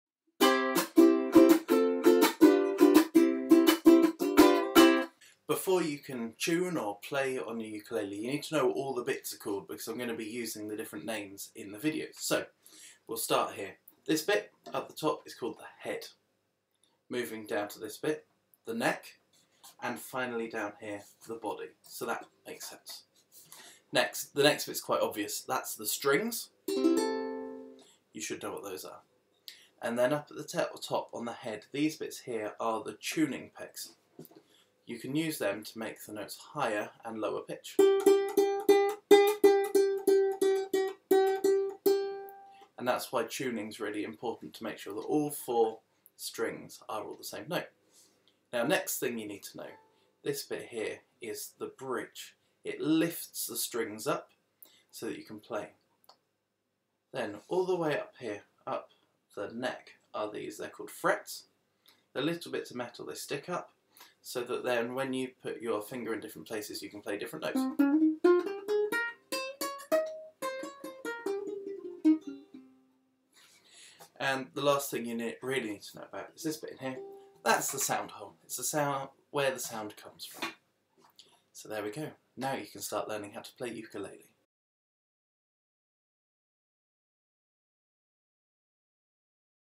Getting Started On The Ukulele
What are all of the different parts called?
Everything you need to know before you start to play!